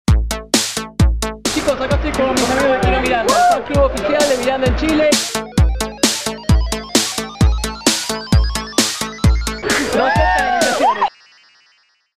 QUIEROmiranda! Fans Club Oficial Chile
Un saludo de Ale para el Fans Club Oficial de Miranda! en Chile

QUIEROmiranda!